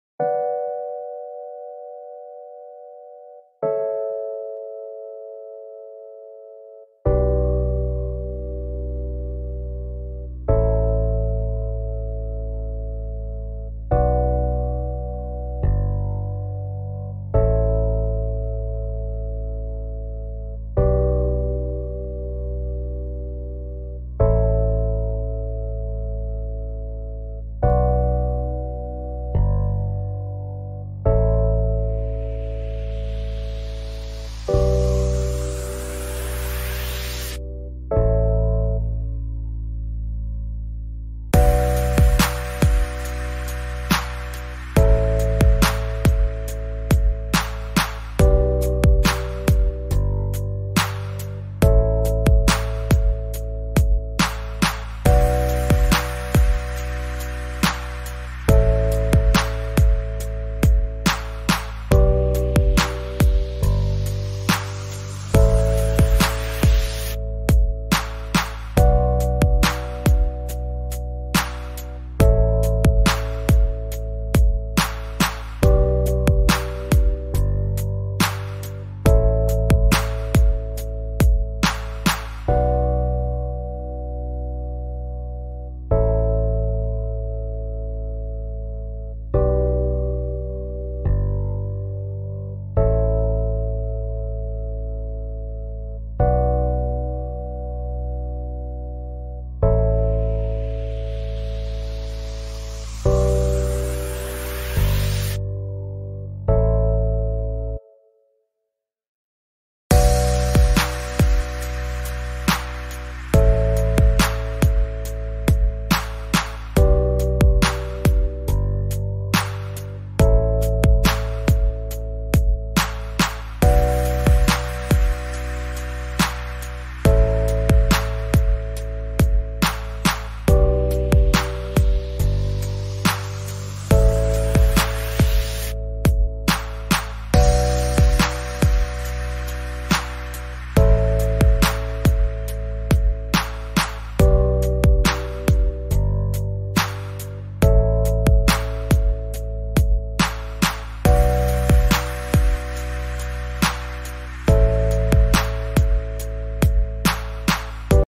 INTEL Core i7 1260P vs INTEL Core i9 13900H Technical Comparison
This video is a processor comparison based on their different features and performance factors between INTEL Core i7 1260P and INTEL Core i9 13900H. Intention of this video is to help choose right processor for you.